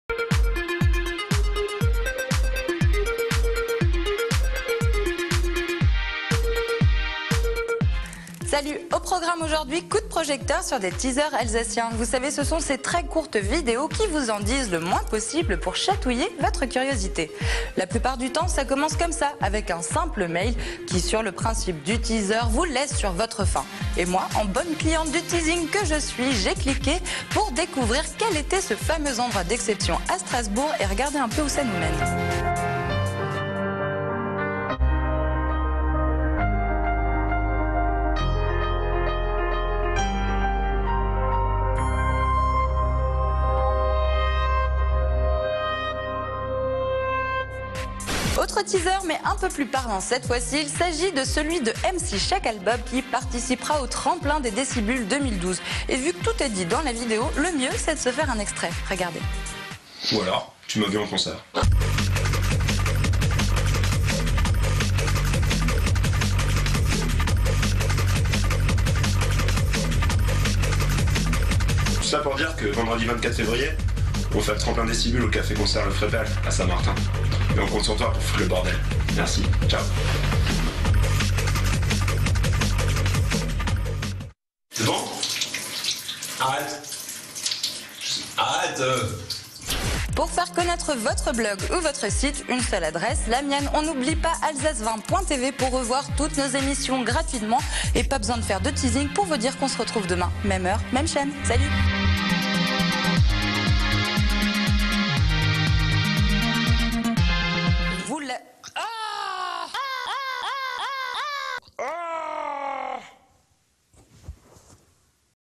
Alsace 2.0 : Les bons plans du net en Alsace (20/03)
Sites webs/blogs, copiez cette vidéo avec le bouton "Intégrer" !
 Morgane fait le point sur les bons plans du net en Alsace.